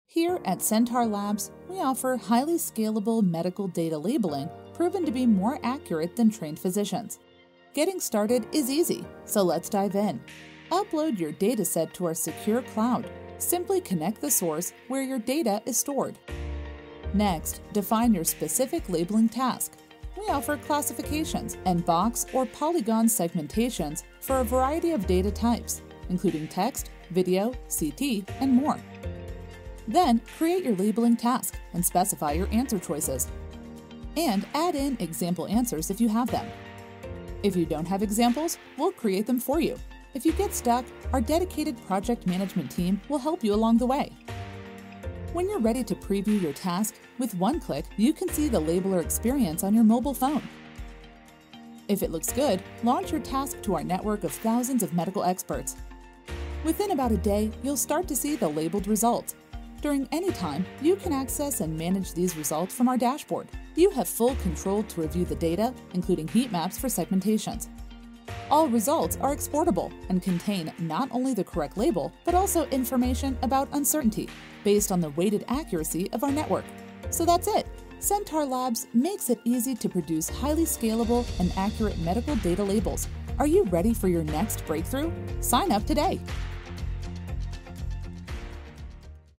Centaur Labs Medical Data Labeling Demo
Using a network of medical experts Centaur Labs' novel approach combines collective intelligence and performance-based incentives to enable industry-leading accuracy of medical data labeling at scale.

In this video, learn how Centaur Labs makes it easy to generate customizable, medical data annotations (classification, segmentation, etc.) with our custom portal.